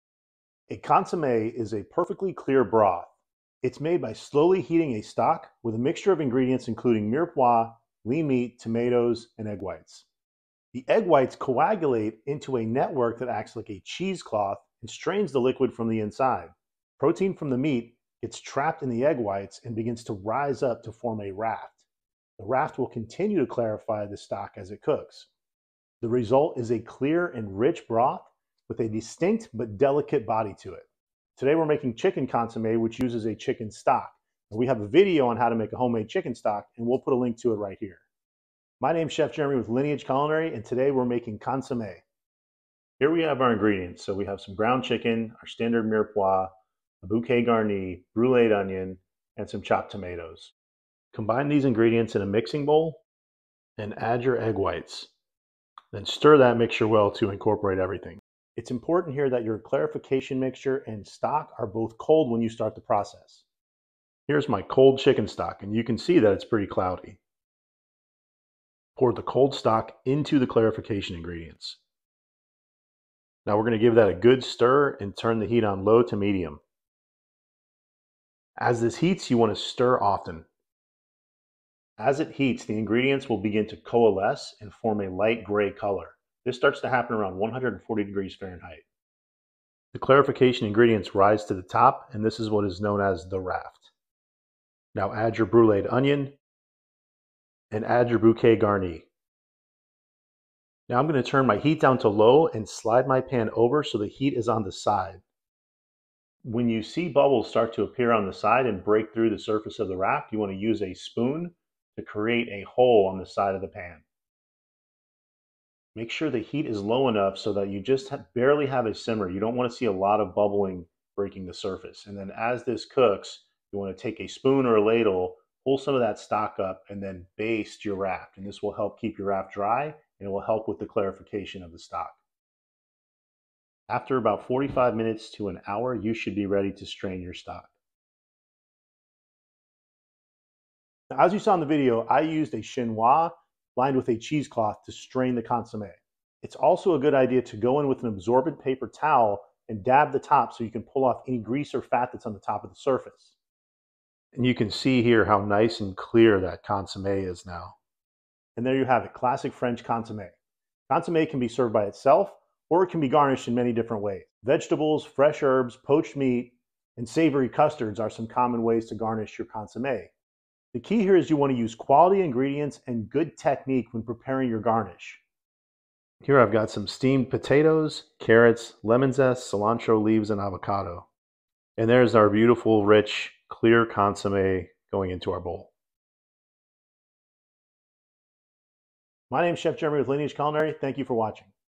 Consomme youtube video
Introduction: French cuisine is renowned for its elegance and complexity, and classic French consommé is a perfect example of this culinary artistry. Consommé is a clear, flavorful soup that serves as a foundation for many French dishes. Made from rich stock and enhanced with aromatic mirepoix, this consommé recipe is a staple in French cooking. Follow this step-by-step guide to create a refined and delicious consommé that will impress even the most discerning palates.